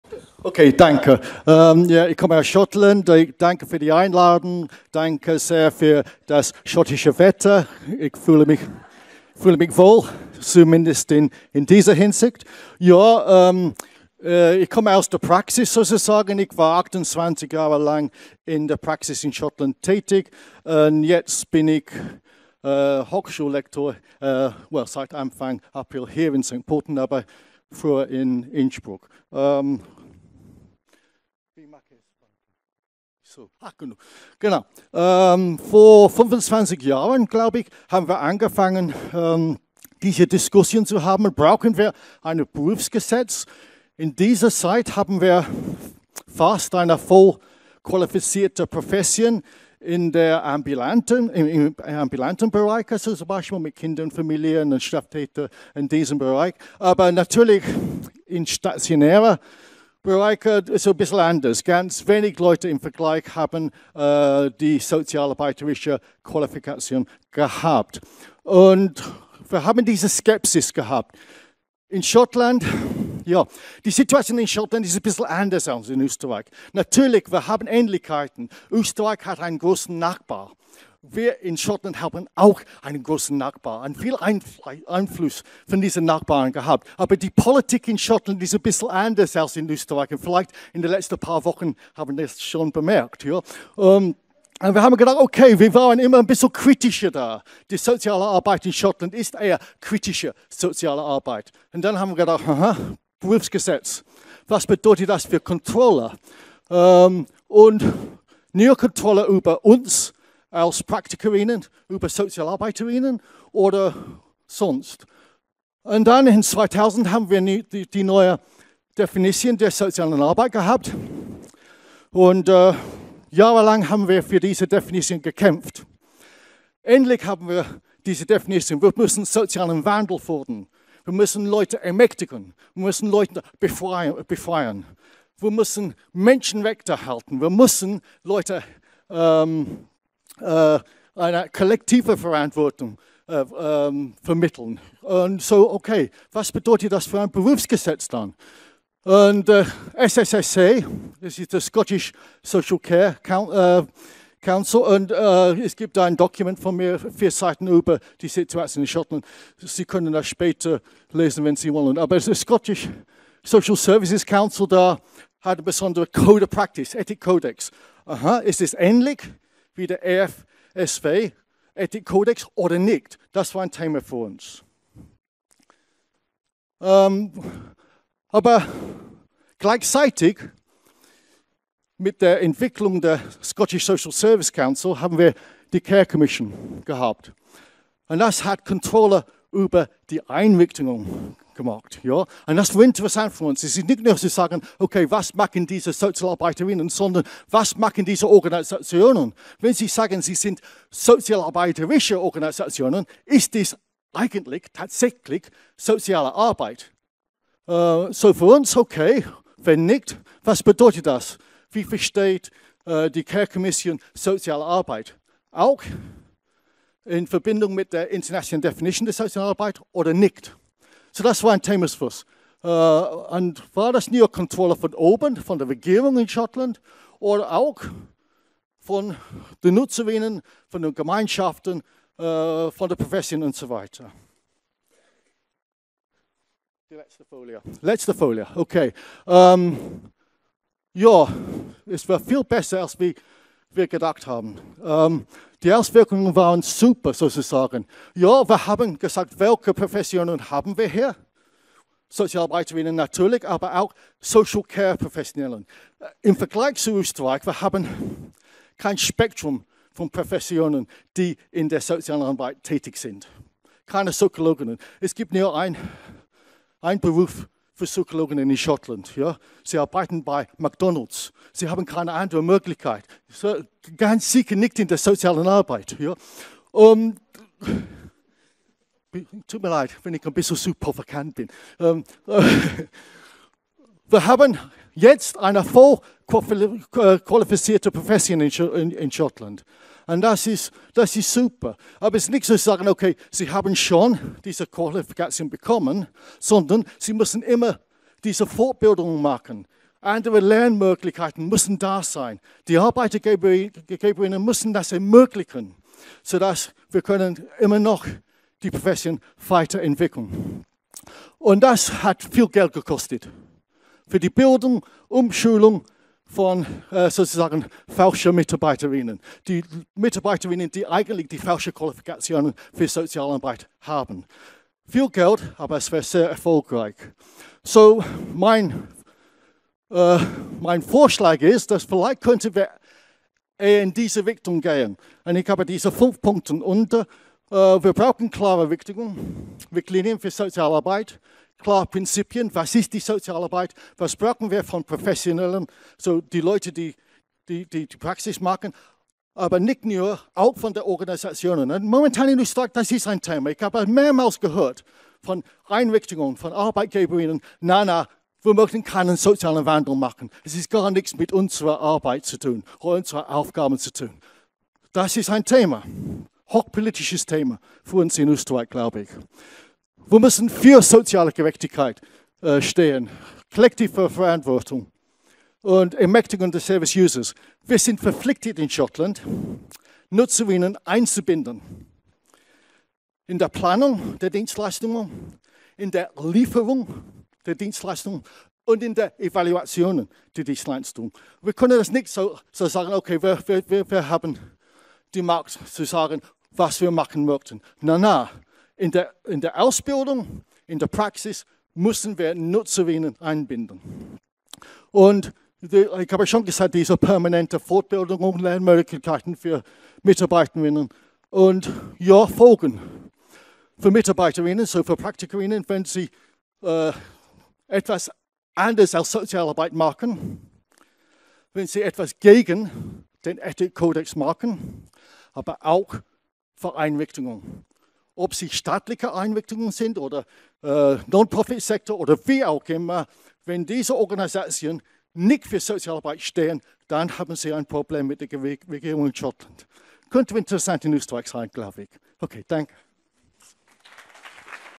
K. Brown: Enhancing Quality – Praktische Auswirkungen berufsgesetzlicher Regelungen in Schottland
Kevin Brown, FH St. Pölten

Social Work Science Day 2015
Soziale Arbeit: Fit für die Zukunft?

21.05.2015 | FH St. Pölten

Enquete: KlientInnen - Professionals - Anstellungsträger: Wer braucht ein Berufsgesetz?